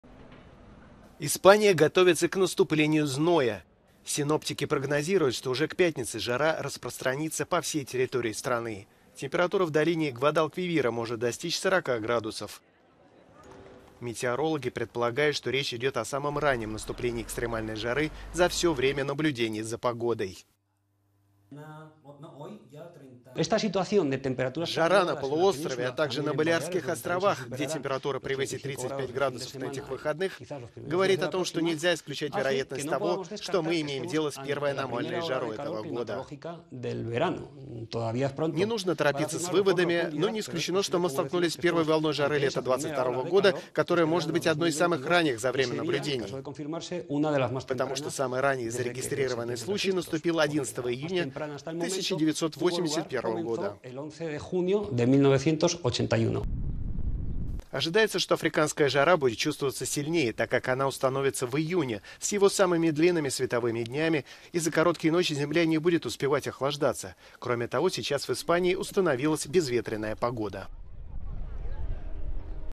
Испанцы готовятся к экстремальной жаре
Температура в долине Гвадалквивира может достичь 40 градусов.